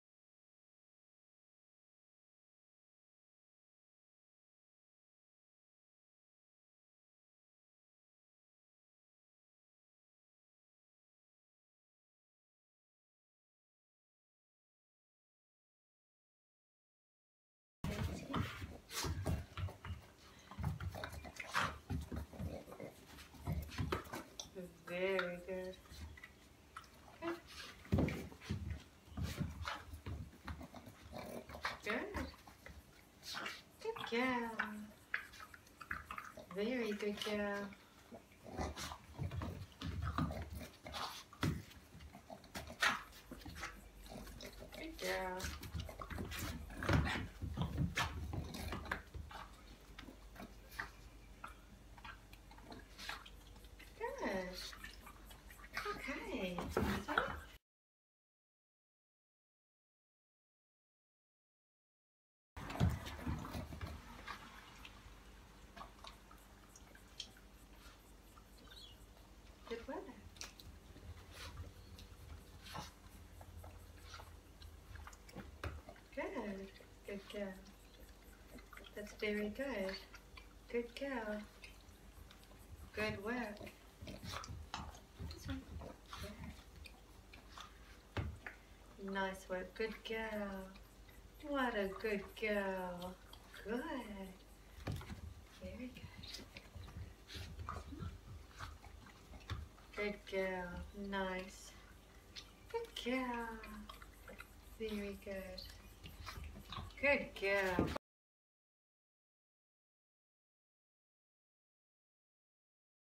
Over enthusiastic dogs on conditioning equipment
Controlling over-enthusiastic dogs on equipment can be challenging.  Most of the time the struggle to learn body awareness because they are too frantic being lured, using a bone broth pouch can calm them down enough to be safe and help them learn.